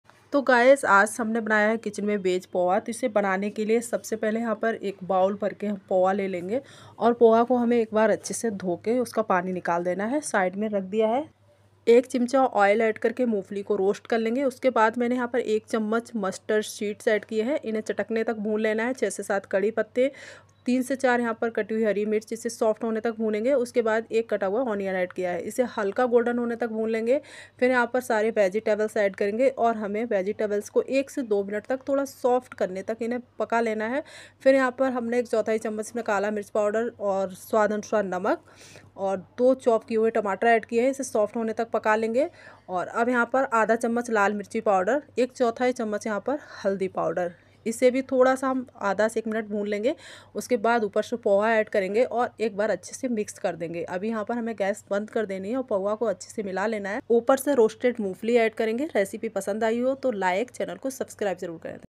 वेज पोहा अगर एक बार ऐसे बना लिए तो बार-बार बनाएंगे | वेज पोहा रेसिपी | Veg poha recipe
Breakfast poha recipe, cooking videos,

Ingredients

​Poha.                          1-bowl
​Mustard seeds.             1-tsp
​Chopped Onion.         1-nug
​Green chilli.                   3 to 4
​Curry patta.                  7 to 8
​Black paper powder.    1/3-tsp
​Chopped tomato.          2-nug
​Chopped beans.             1.5-Ttsp
​Chopped carrot.         2-Ttsp
​Chopped capsicum.   1.5-Ttsp
​Roasted mungfali.      1.5-Ttsp
​Salt to test